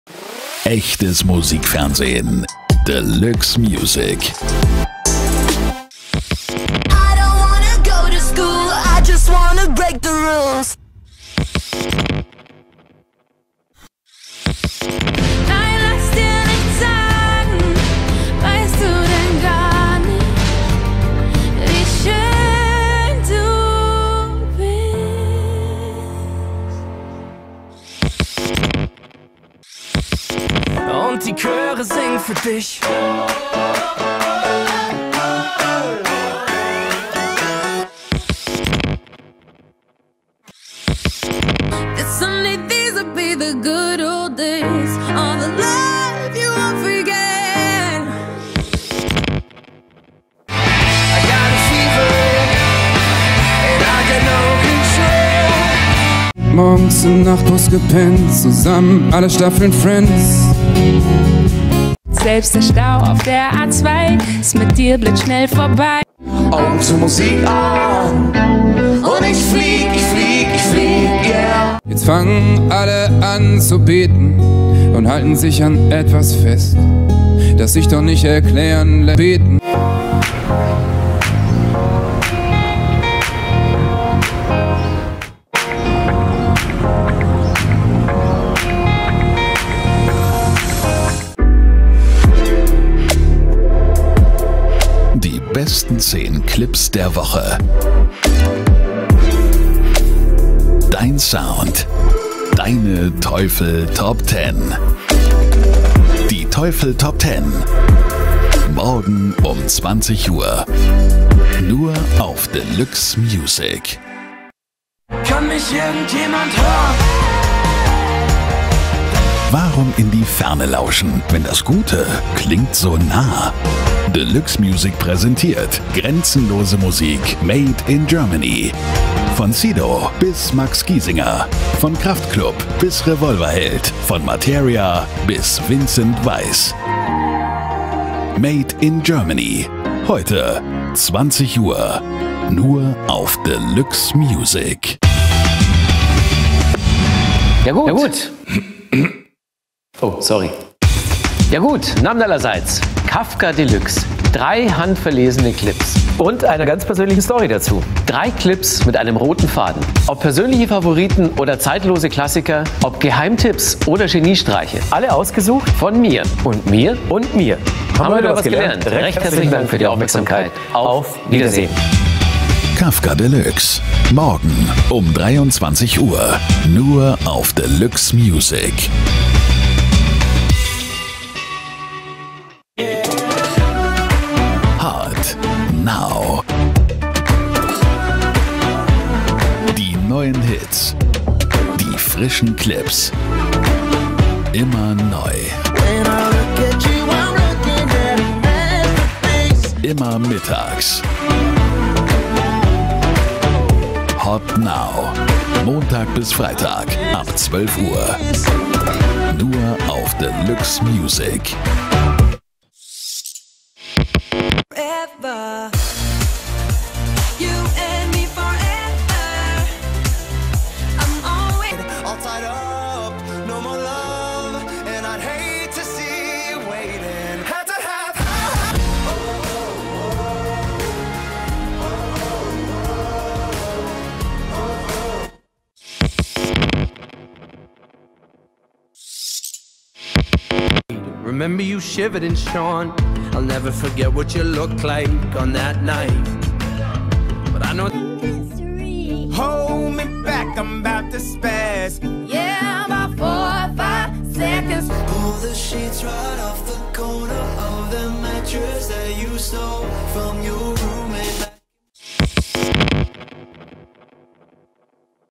Deluxe Music - Idents, Promos and Bumpers (2019)
Zum heutigen Relaunch eine kleiner Zusammenschnitt der neuen Idents, Promos und Bumper auf Deluxe Music.

Aufgenommen am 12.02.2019 via 1&1 Digital-TV.